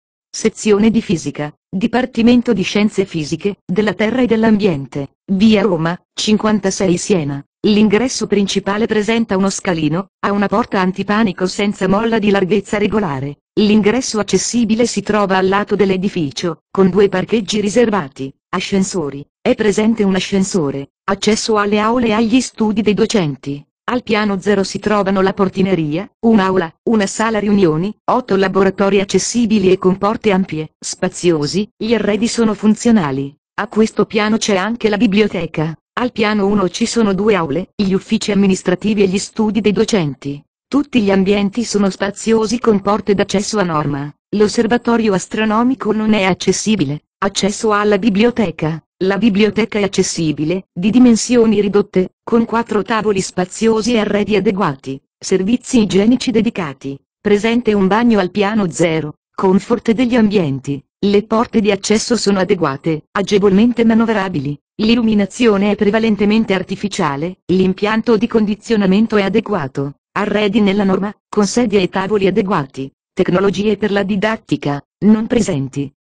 Accessibilità del complesso didattico San Niccolò (sezione di Fisica) - file audio
Il video integra i contenuti delle pagine web dedicate all'Accessibilità delle sedi didattiche dell'Università di Siena a cura dell'ufficio Accoglienza disabili e servizi DSA.